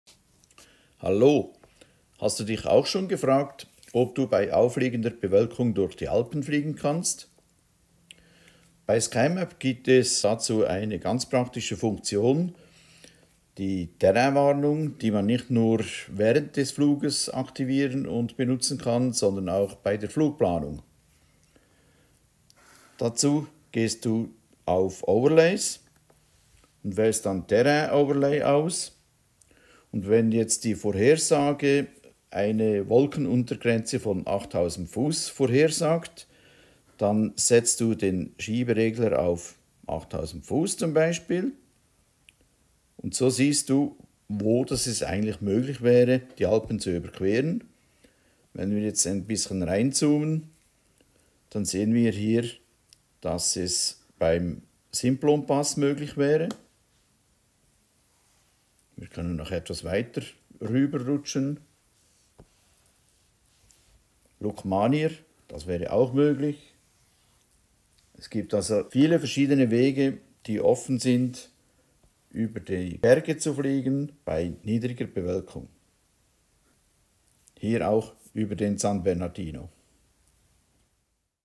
Sky-Map Terrain Warnung für die Flugplanung
Die Funktion "Terrain Overlay" ist bei Sky-Map seit Jahren eine hilfreiche Funktion in der Flugplanung.